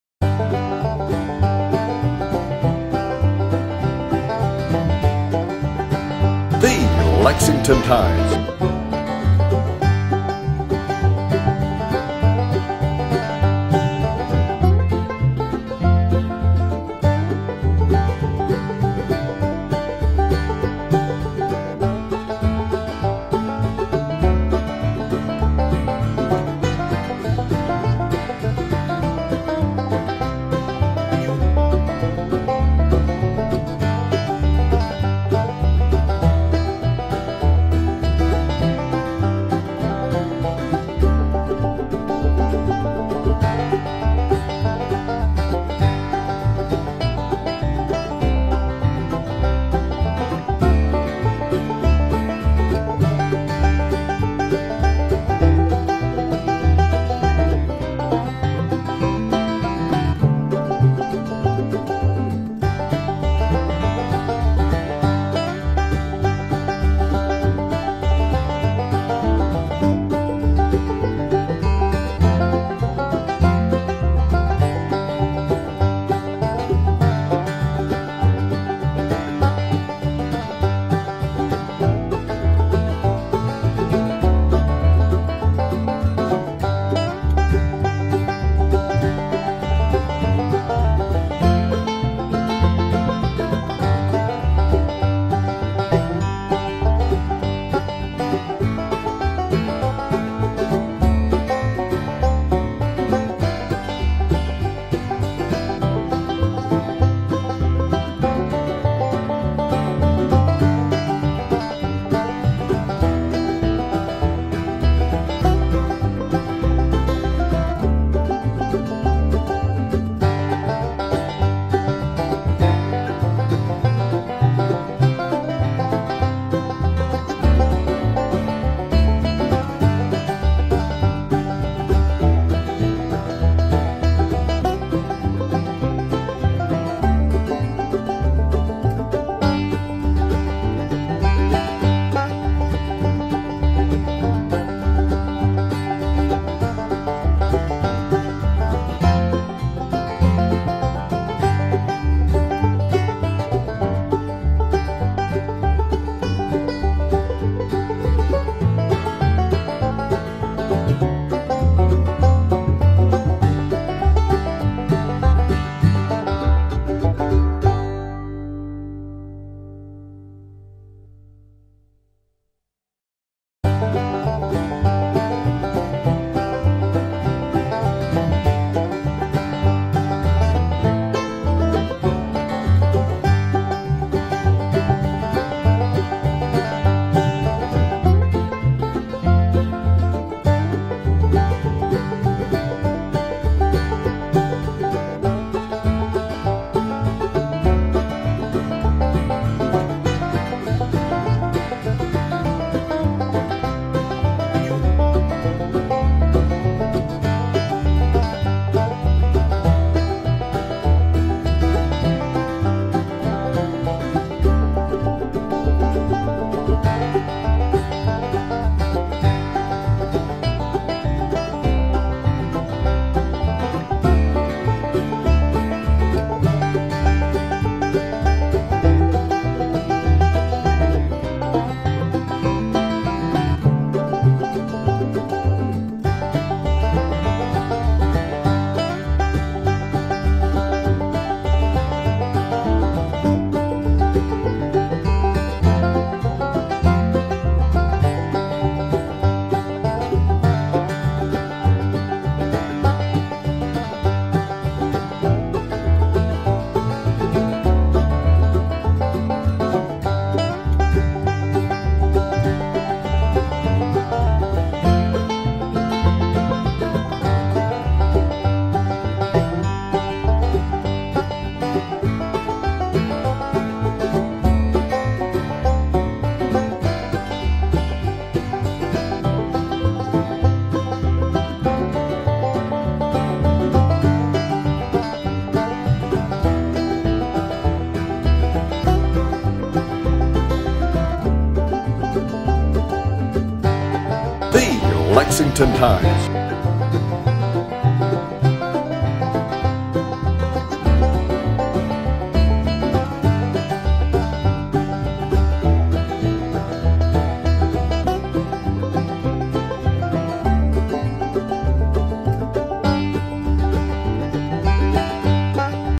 Map of Fayette County, Kentucky in1891 (4K w/ Land Owner Names)
4K overview of a 1891 map of Fayette County, Kentucky

Wallis, W. R. (1891) Map of Fayette County, Kentucky. [Lexington, Ky.: s.n] [Map] Retrieved from the Library of Congress,